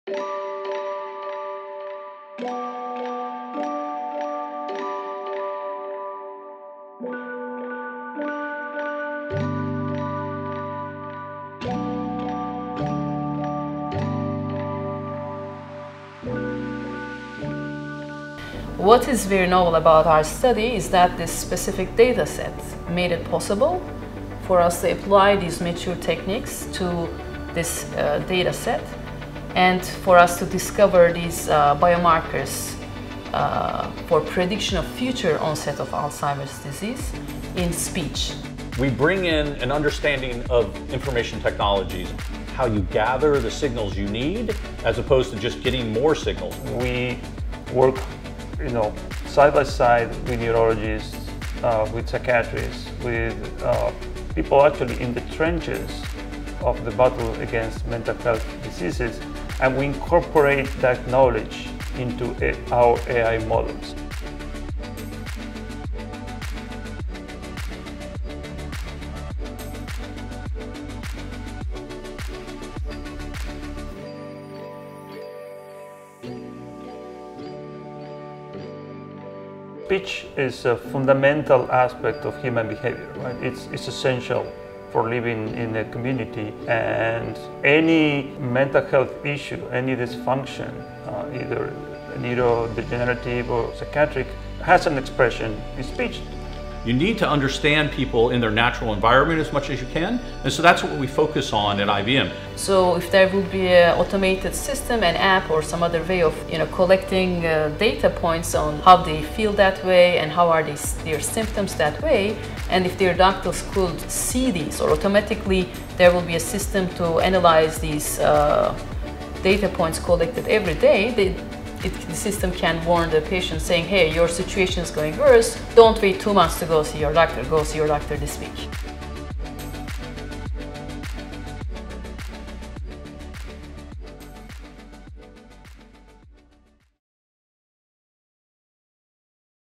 What's New in AI? Helping Predict Alzheimer's
A new AI model, developed by IBM Research and Pfizer, uses short, non-invasive speech samples to help predict the eventual onset of Alzheimer's disease within currently cognitively healthy individuals.